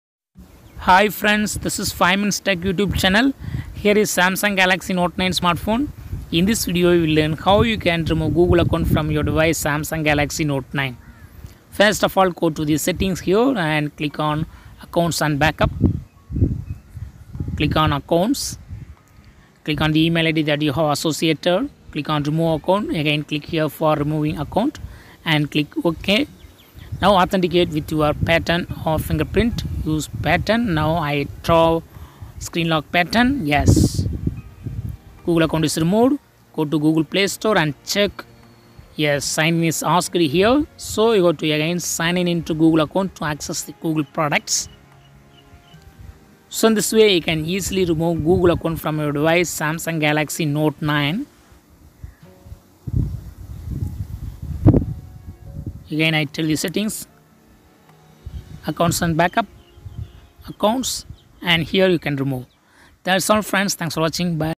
How to remove Google account from Samsung Galaxy Note 9
Samsung Galaxy Note 9 Google account remove settings: Learn here how to remove Google account from Samsung Galaxy Note 9 smartphone